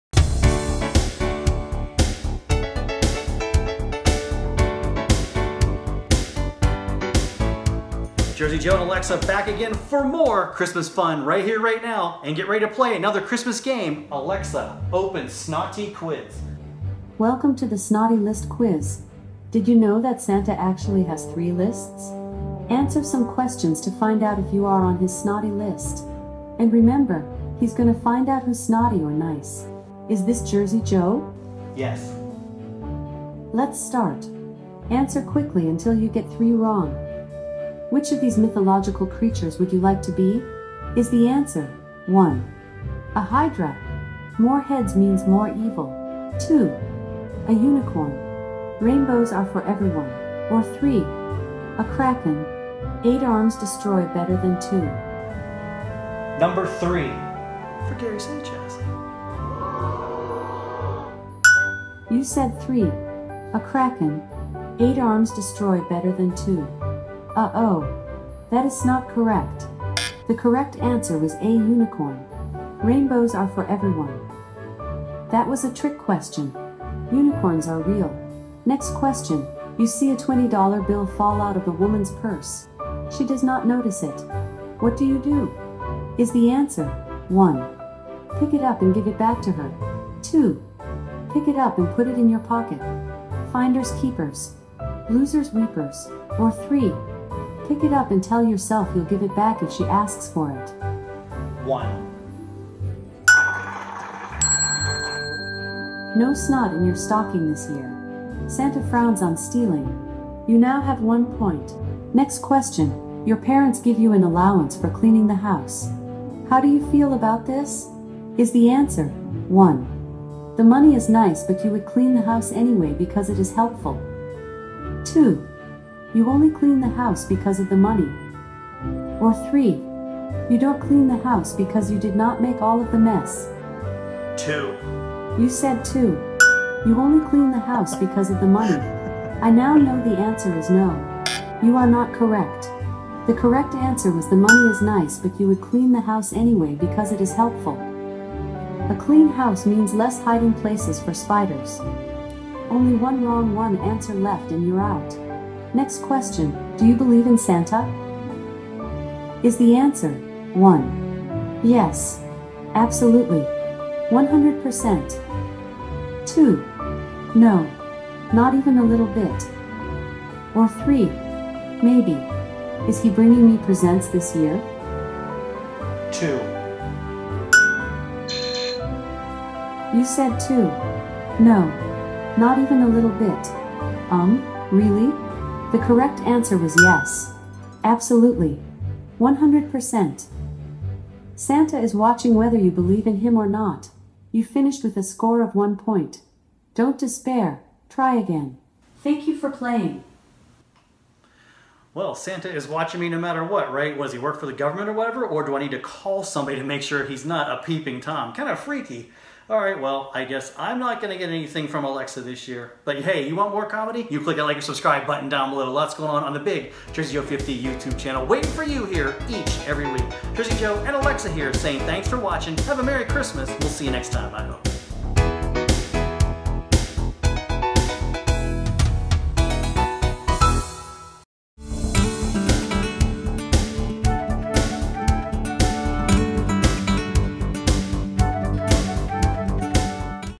Alexa Christmas Snotty List Quiz [Jersey Joe # 488]
Should Santa bring you a Christmas present this year?  Play along as Jersey Joe and Amazon Alexa take the Christmas Snotty List Quiz!

KICKING BACK WITH JERSEY JOE # 488